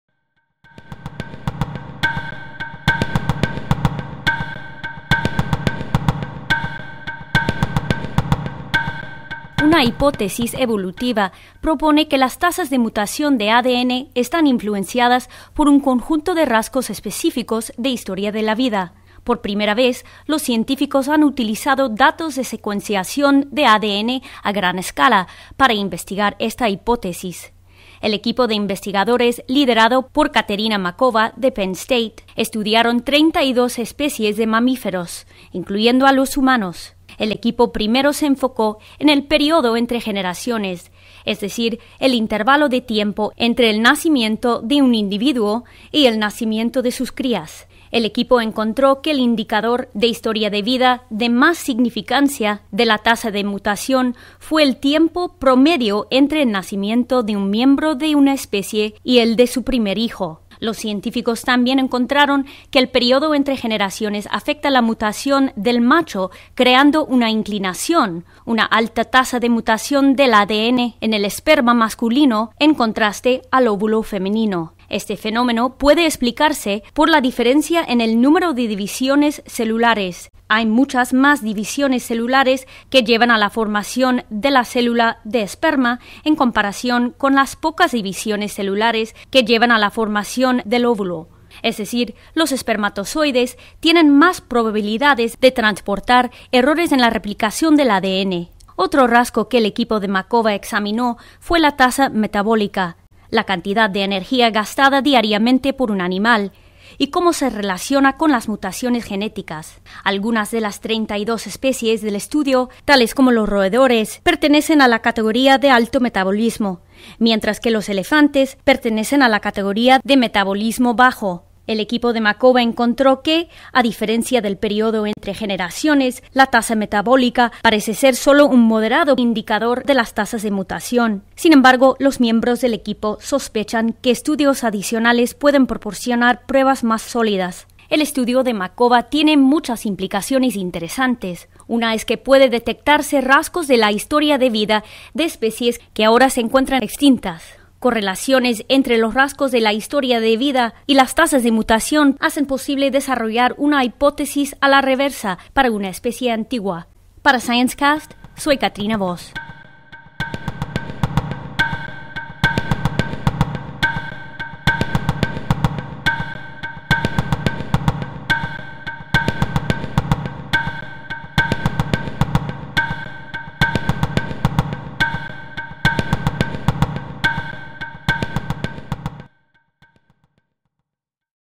ScienceCast en Español: Los rasgos de historia de vida y las tasas de mutación del ADN
Una hipótesis evolutiva propone que las tasas de mutación de ADN están influenciadas por un conjunto de rasgos específicos de historia de la vida. Por primera vez, los científicos han utilizado datos de secuenciación de ADN a gran escala para investigar esta hipótesis. (9 noviembre 2011)